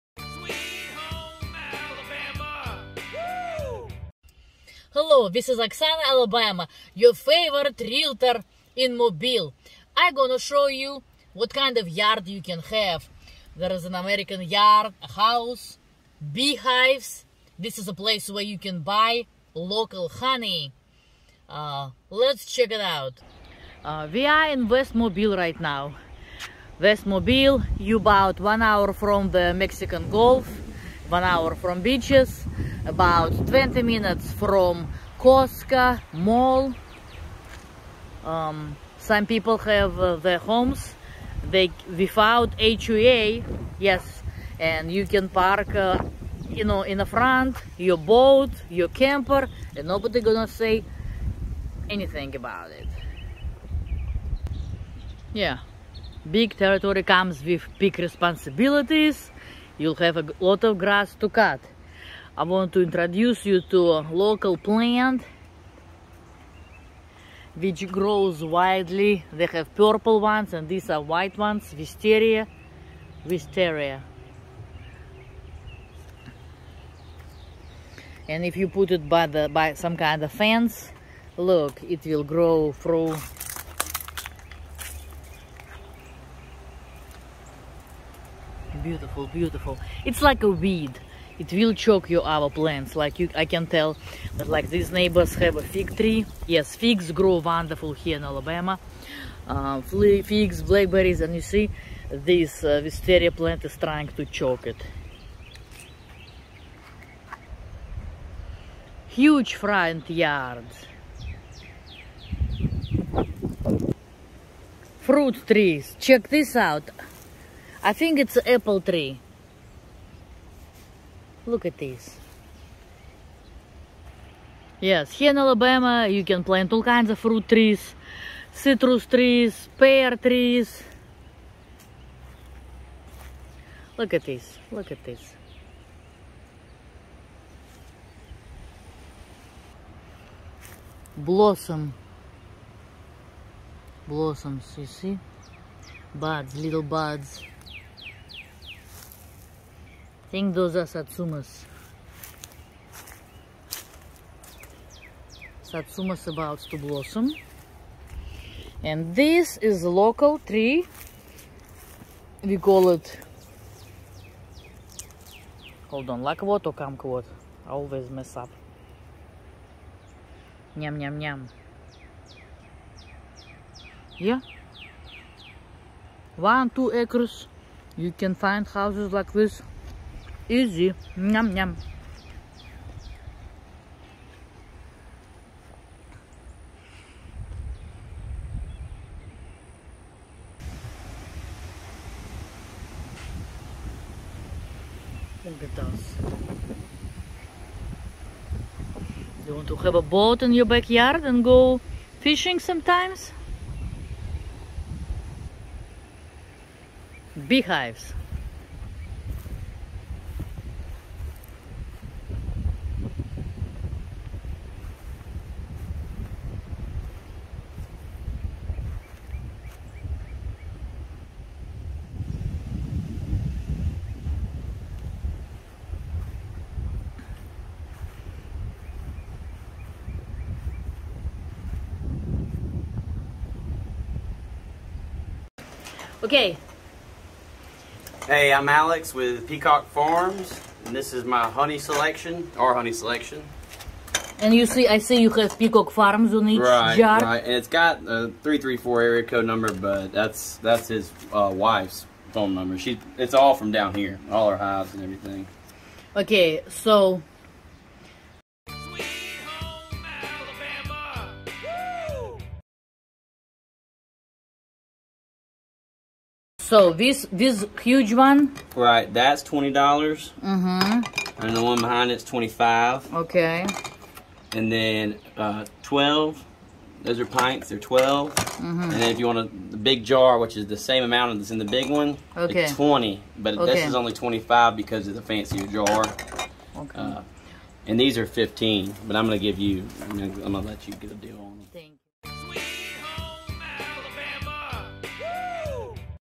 🏵🌹ALABAMA YARDS BEE HIVES and HONEY🐝🐝🐝
Meet local beekeeper and check out his yard with fruit trees and beehives.